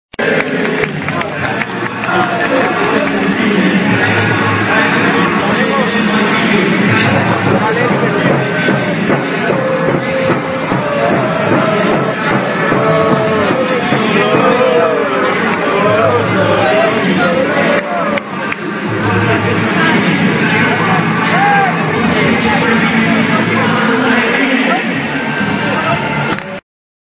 SCM U.CRAIOVA -- Csm Bucuresti 78-71 27/02/2013
Scm U.Craiova a obtinut o noua victorie in Sala Polivalenta din Craiova in fata publicului .
Care au dus echipa spre o noua victorie ! Craiova Bate Pentru Tine